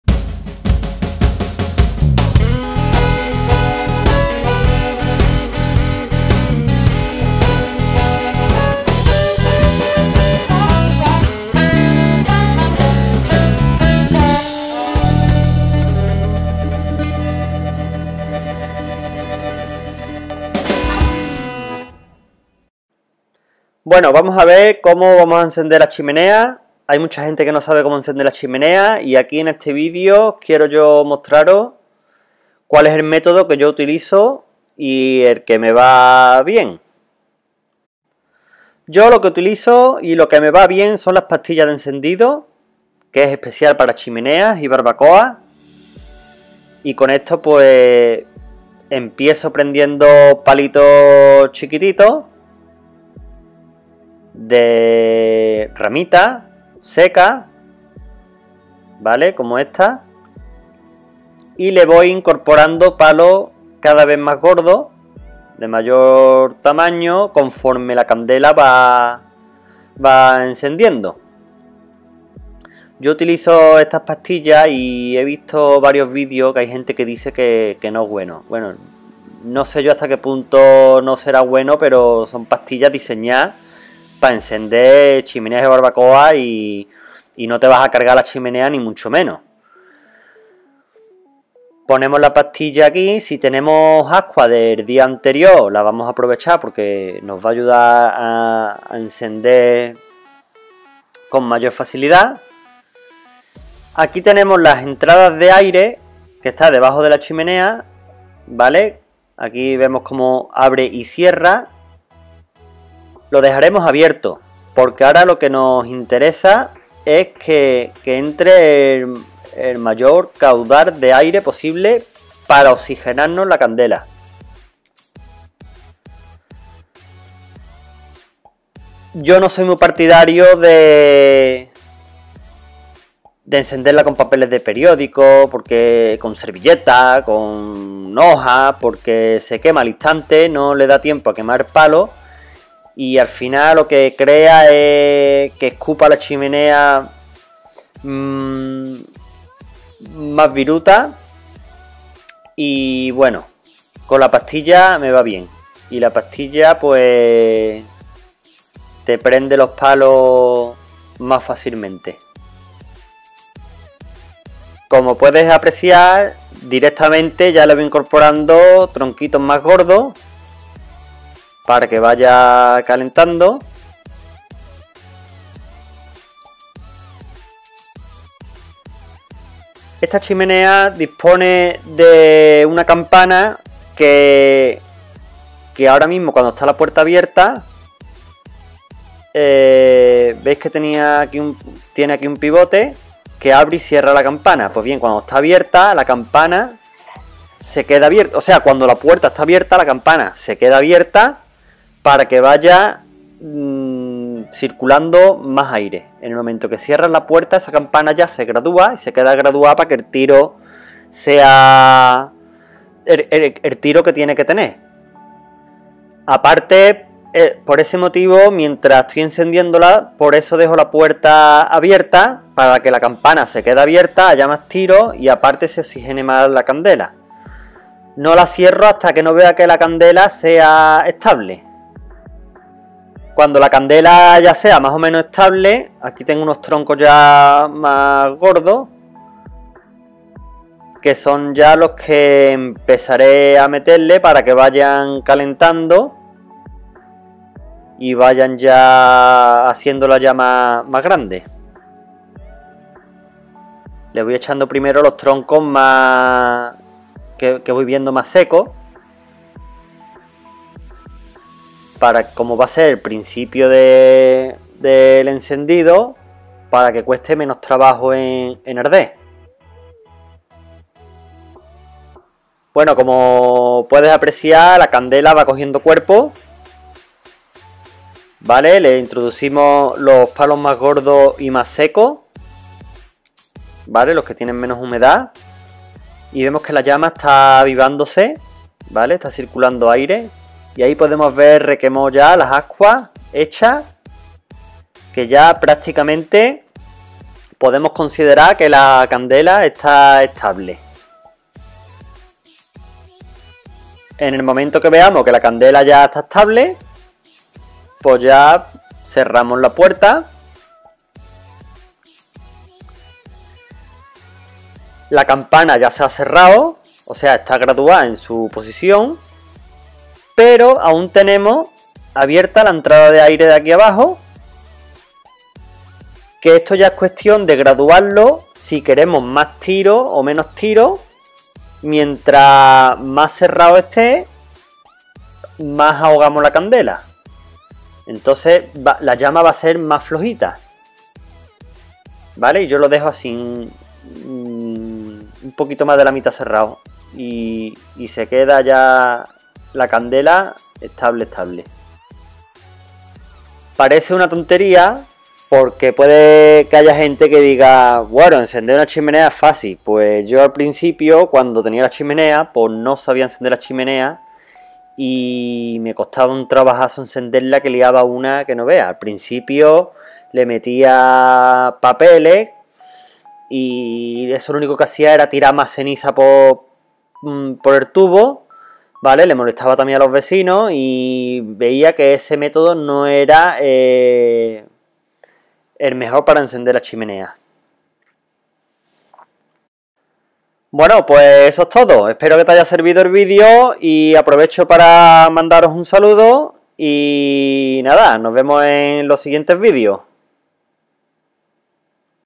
COMO ENCENDER UNA CHIMENEA
Parece sencillo encender una chimenea asta que te pones a encenderla, por eso he decidido hacer este video, para que puedas encenderla de forma sencilla y rápida.